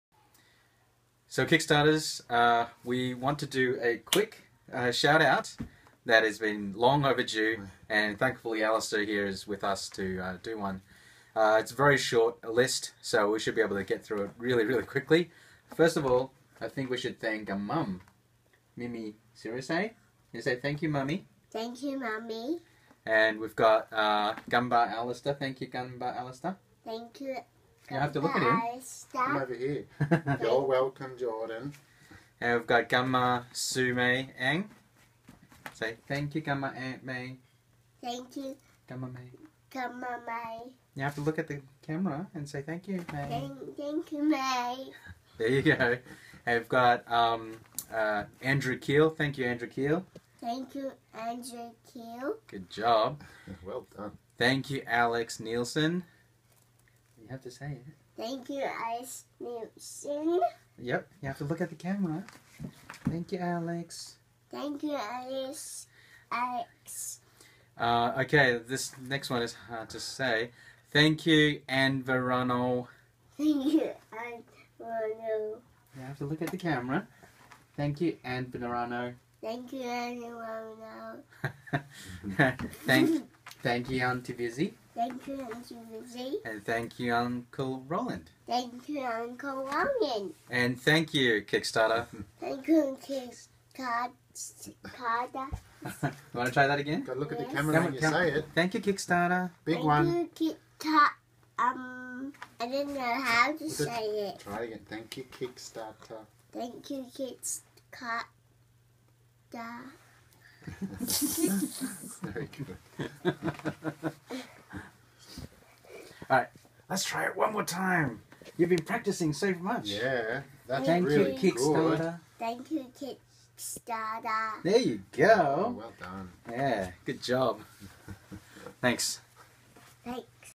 Shout-outs from Jordan
Jordan giving shout-outs to a few of Rise to Power's Kickstarter backers.